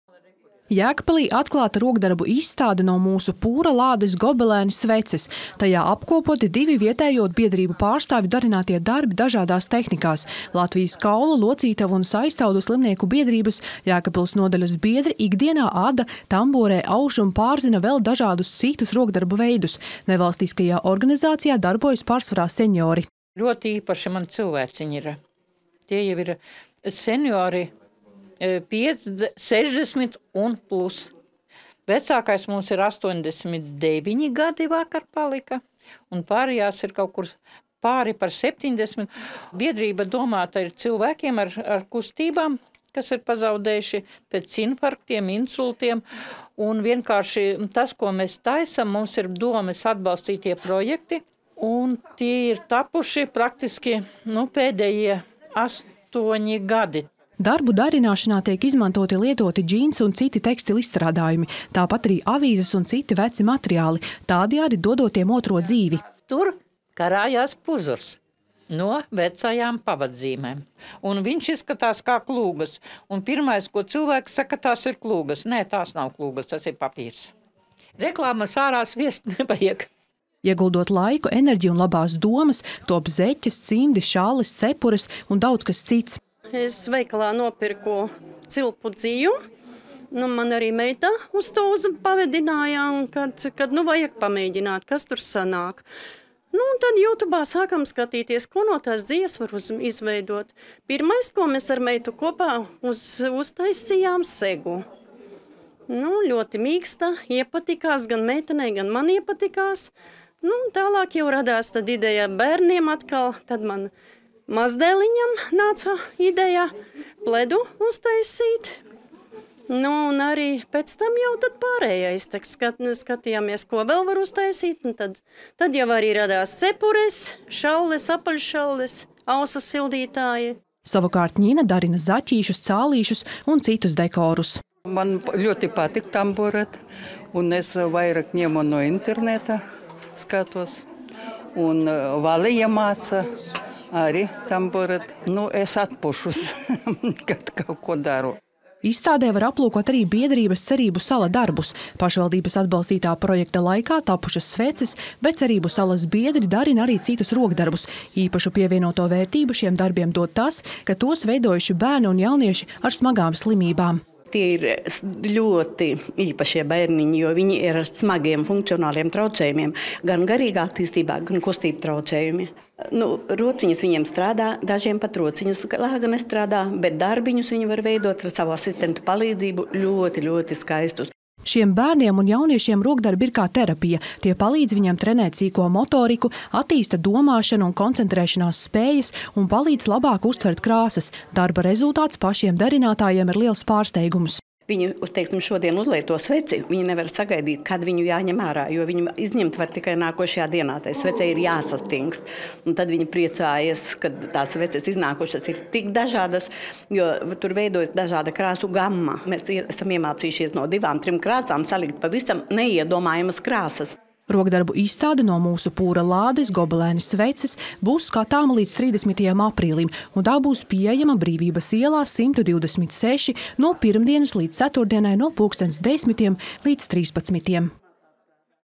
Jēkabpils biedrības apvieno rokdarbus izstādē “No mūsu pūra lādes – gobelēni, sveces”
Jēkabpilī atklāta rokdarbu izstāde “No mūsu pūra lādes – gobelēni, sveces”. Tajā apkopoti divu vietējo biedrību pārstāvju darinātie darbi dažādās tehnikās. Latvijas Kaulu, locītavu un saistaudu slimnieku biedrības Jēkabpils nodaļas biedri ikdienā ada, tamborē, auž un pārzina vēl dažādus citus rokdarbu veidus. Nevalstiskajā organizācijā darbojas pārsvarā seniori.

“Mums ir ļoti īpaši cilvēciņi,” stāsta nodaļas vadītāja Valija Cīrule. “Tie ir seniori 60 plus. Vecākajam biedram ir 89 gadi, pārējiem ir pāri 70. Biedrība ir domāta cilvēkiem, kuri zaudējuši kustības pēc infarkta vai insulta. Mēs taisām darbus, īstenojot domes atbalstītus projektus. Tie ir tapuši pēdējo astoņu gadu laikā.”

Darbu darināšanā tiek izmantoti lietoti džinsi un citi tekstilizstrādājumi, tāpat arī avīzes un citi veci materiāli, tādējādi dodot tiem otro dzīvi.

“Piemēram, ir puzurs, kas veidots no vecajām pavadzīmēm,” stāsta Valija Cīrule. “Izskatās kā klūgas, un pirmais, ko cilvēki saka, ir, ka tās ir klūgas. Nē, tās nav klūgas, tas ir papīrs. Reklāmas nevajag sviest ārā.”

Ieguldot laiku, enerģiju un labās domas, top zeķes, cimdi, šalles, cepures un daudz kas cits.

Biedrības pārstāve Austra stāsta: “Es veikalā nopirku cilpu dziju, mani arī meita uz to vedināja, ka vajag pamēģināt, kas tur sanāks. Tad youtube sākām skatīties, ko no tās dzijas var izveidot. Pirmais, ko mēs ar meitu kopā uztaisījām, bija sega. Sanāca ļoti mīksta, iepatikās gan meitenei, gan man. Tad mazdēliņam nāca ideja pledu uztaisīt. Skatījāmies, ko vēl var uztaisīt. Tad jau radās cepures, šalles, apaļšalles, ausu sildītāji.”

Savukārt Ņina darina zaķīšus, cālīšus un citus dekorus: “Mn ļoti patīk tamborēt, es ņemu idejas no interneta, arī Valija iemāca. Kad kaut ko daru, es atpūšos.”

Izstādē var aplūkot arī biedrības “Cerību sala” darbus. Pašvaldības atbalstītā projekta laikā tapušas sveces, bet “Cerību salas” biedri darina arī citus rokdarbus. Īpašu pievienoto vērtību šiem darbiem dod tas, ka tos veidojuši bērni un jaunieši ar smagām slimībām.

“Viņi ir ļoti īpaši bērniņi, jo viņi ir ar smagiem funkcionāliem traucējumiem – gan garīgās attīstības, gan kustību traucējumiem. Dažiem pat rociņas lāga nestrādā, bet darbiņus viņi ar savu asistentu palīdzību var veidot ļoti skaistus,” stāsta biedrības “Cerību sala” valdes priekšsēdētāja Aija Kvēpiņa.

Šiem bērniem un jauniešiem rokdarbi ir kā terapija – tie palīdz viņiem trenēt sīko motoriku, attīsta domāšanu un koncentrēšanās spējas un palīdz labāk uztvert krāsas. Darba rezultāts pašiem darinātājiem ir liels pārsteigums.

“Viņi uzliek to sveci un nevar sagaidīt, kad būs jāņem ārā, jo to izņemt var tikai nākošajā dienā. Svecei ir jāsastingst. Un tad viņi priecājas, ka tās sveces ir iznākušas tik dažādas. Tur veidojas dažādas krāsu gammas. Mēs esam iemācījušies no divām, trim krāsām salikt pavisam neiedomājamas krāsas,” skaidro Aija Kvēpiņa.

Rokdarbu izstāde “No mūsu pūra lādes – gobelēni, sveces” būs skatāma līdz 30. aprīlim, un tā būs pieejama Brīvības ielā 126 no pirmdienas līdz ceturtdienai no pulksten 10:00 līdz 13:00.

Sandra Paegļkalne